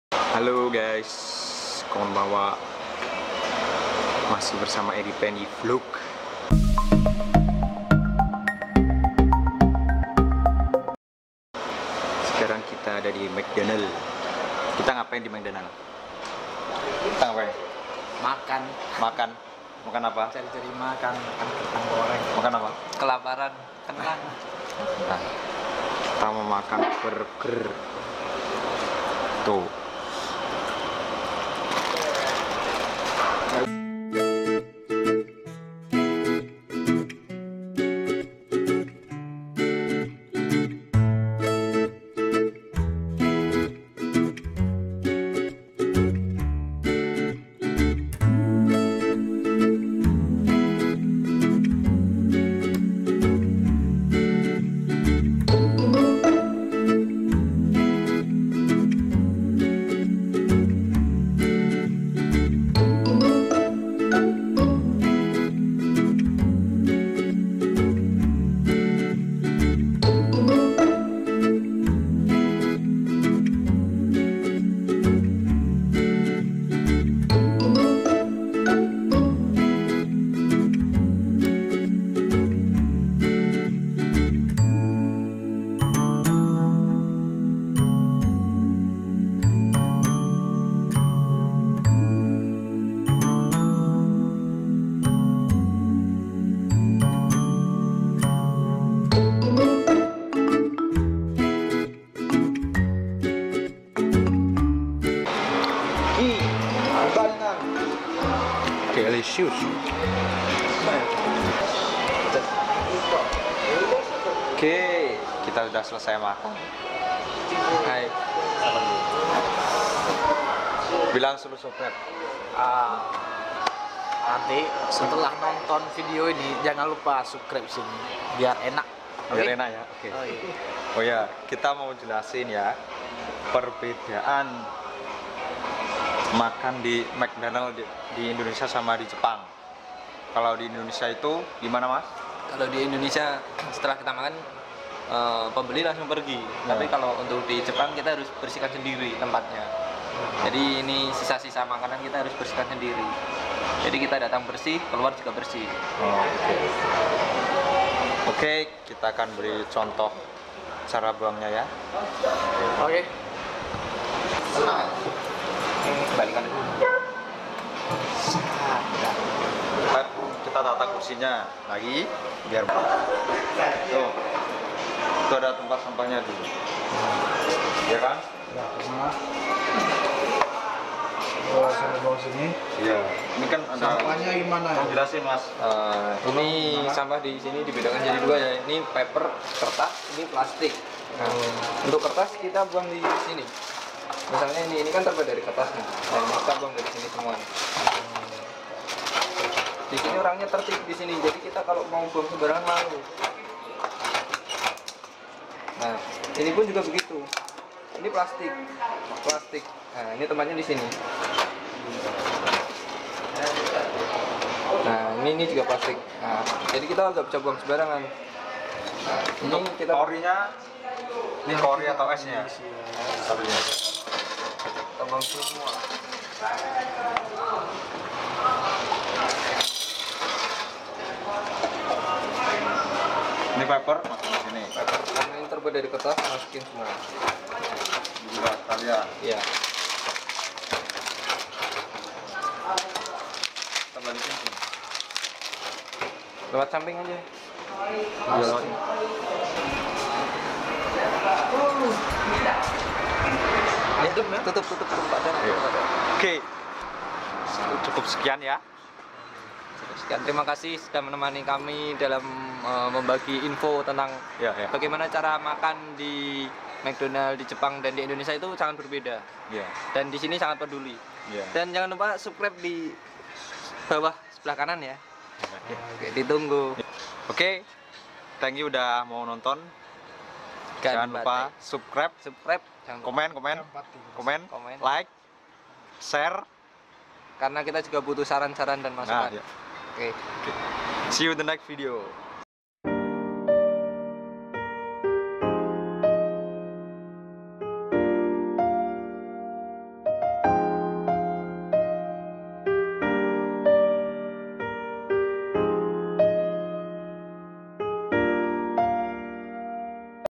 PENTING DI KETAHUI ! Makan Mc Donald Di Jepang (berbeda)
haloo minnasan... kali ini aku bikin vlog perbedaan kebiasaan setelah makan mc donald di jepang, gak penting sih tapi selamat menonton...